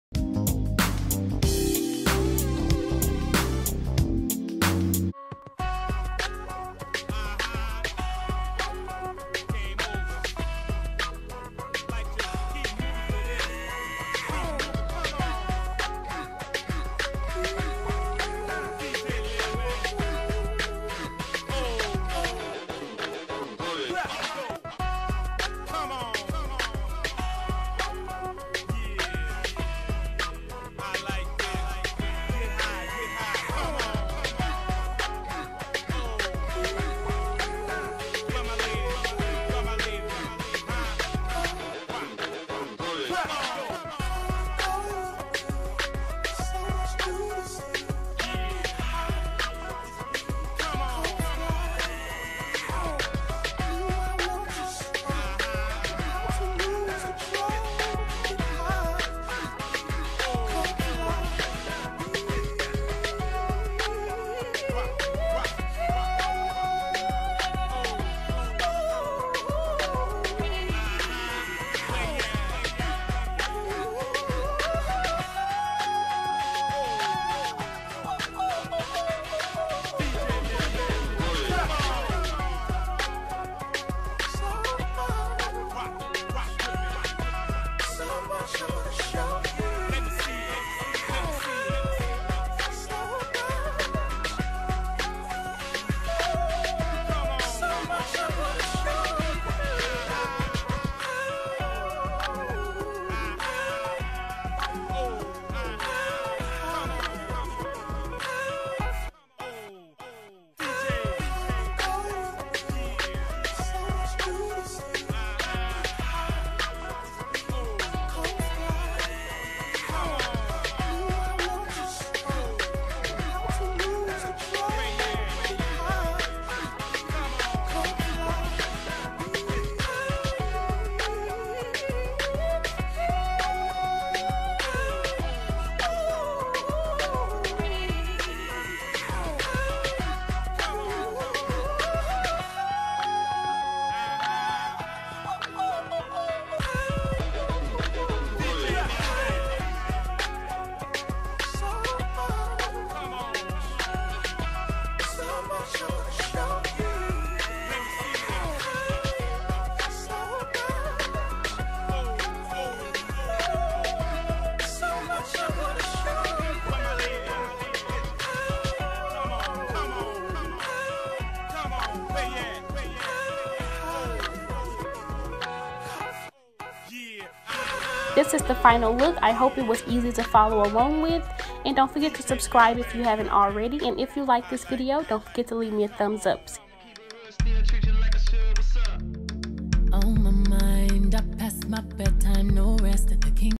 Valentines Day Fun and Flirty Makeup Tutorial
Hey loves, here is another Vday look.
Let me know what you guys think down below in the comments.
What are your plans for Valentines Day?